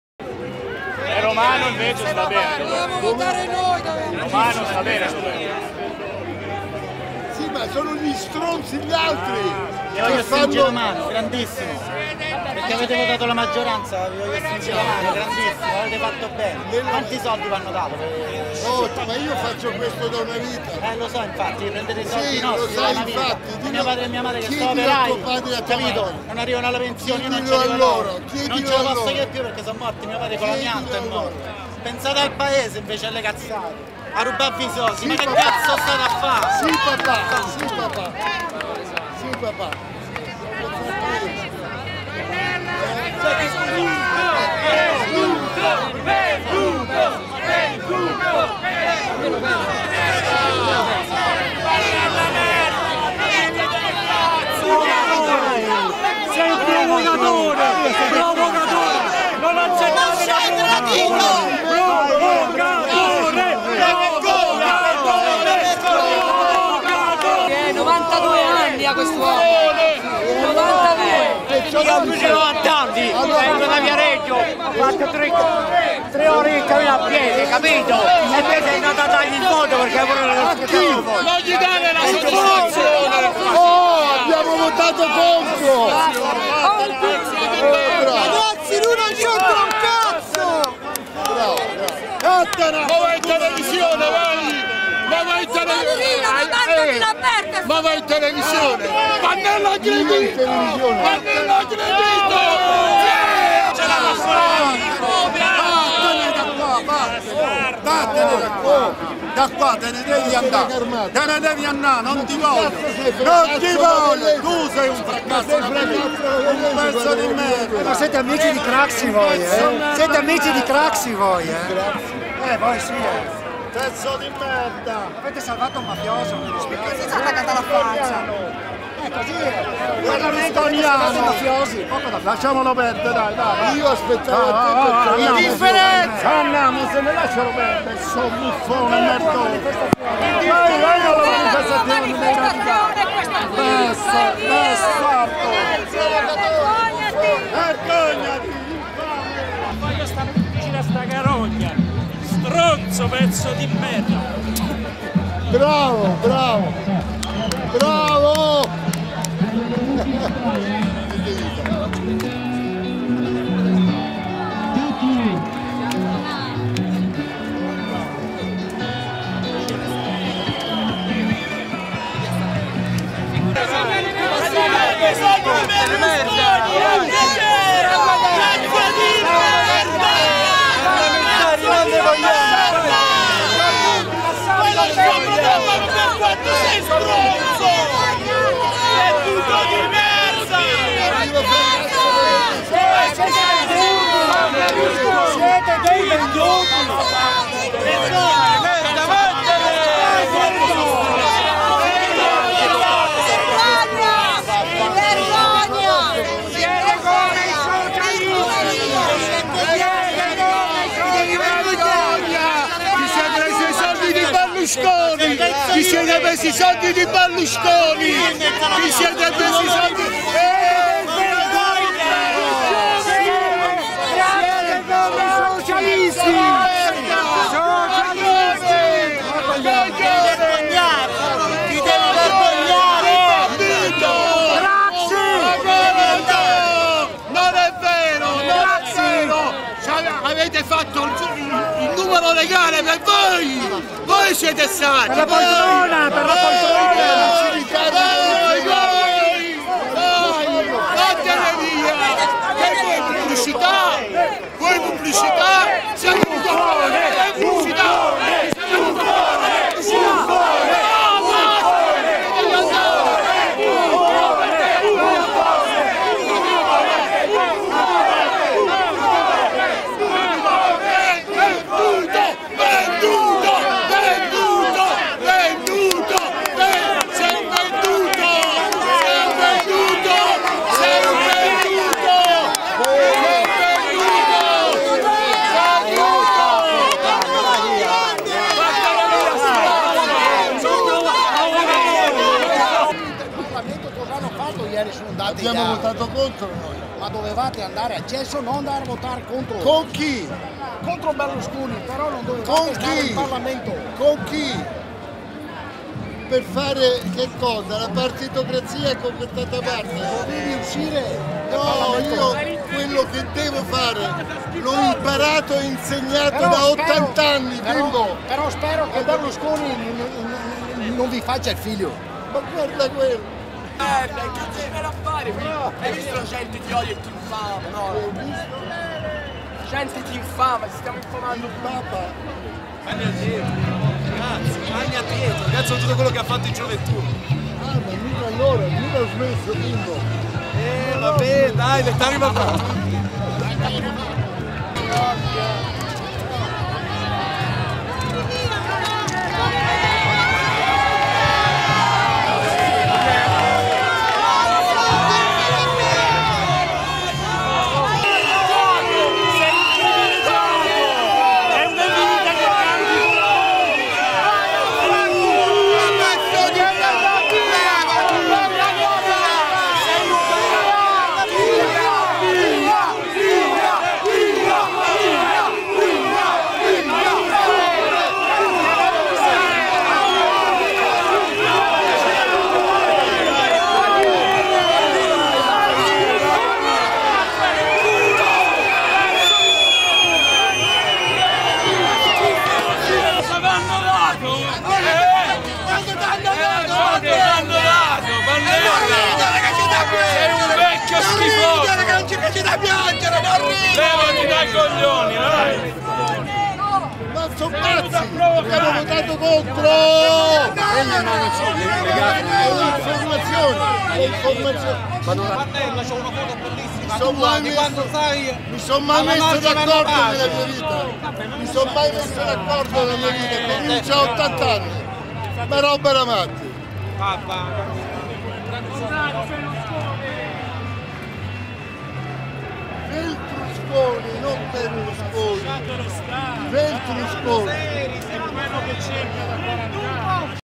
Come il Regime ti cucina gli Indignados
Marco Pannella, da 30 anni impegnato nella denuncia contro il debito, partecipa al corteo di Roma